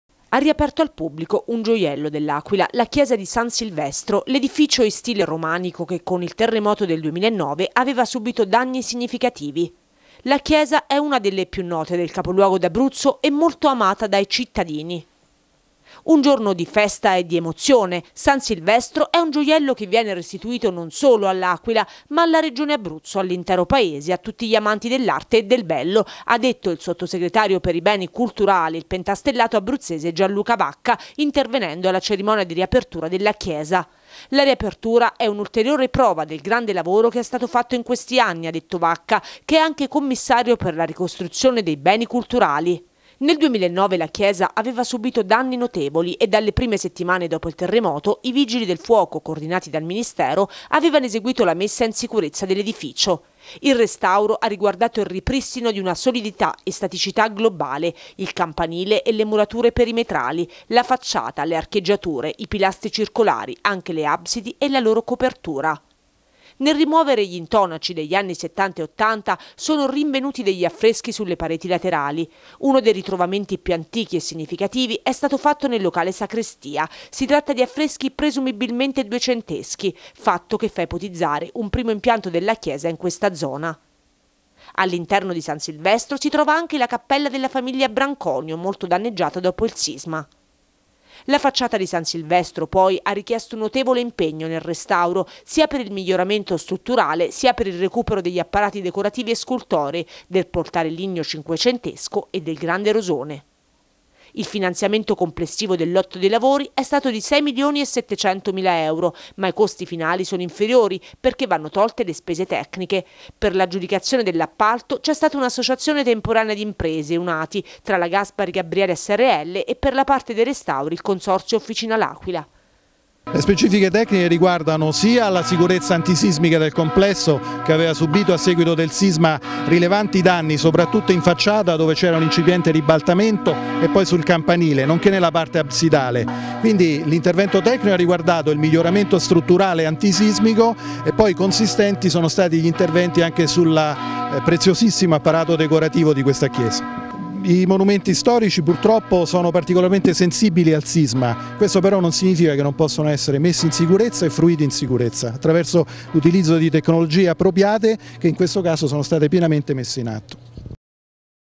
San Silvestro riapre al culto
San Silvestro dopo un lungo e complesso restauro riapre al culto. Una festa per la città che si riappropria di uno dei suoi simboli identitari